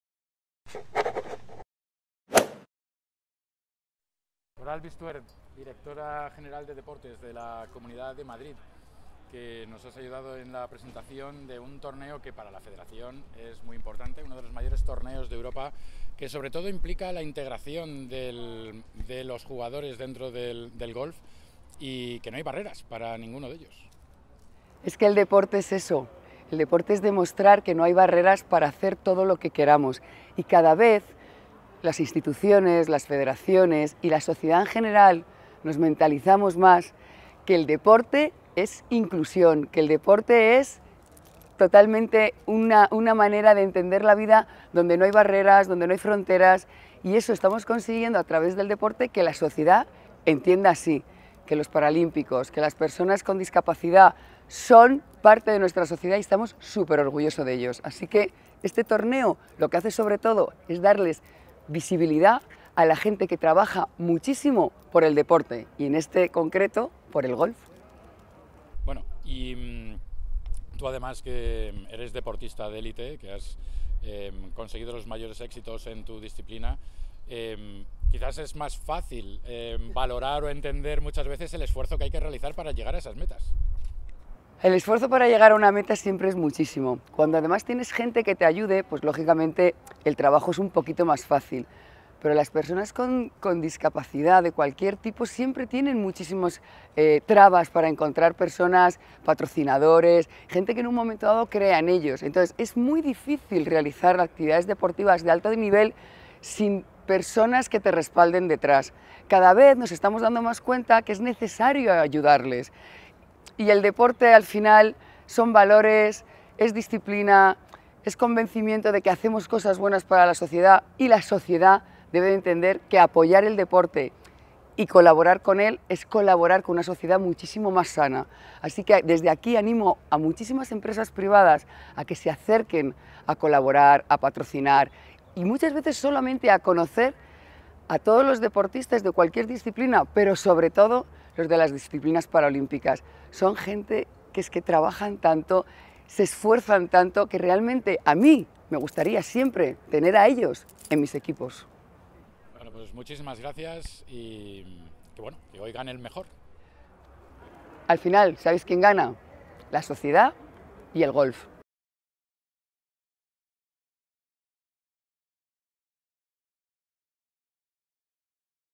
Coral Bistuer: de una deportista a los deportistas.
Coral Bistuer, directora general de deportes de la Comunidad de Madrid, nos acompañó en la presentación del DAIKIN Madrid Open, el mayor torneo de golf adaptado de España que organiza la Federación de Golf de Madrid del 17 al 19 de septiembre en el Centro Nacional.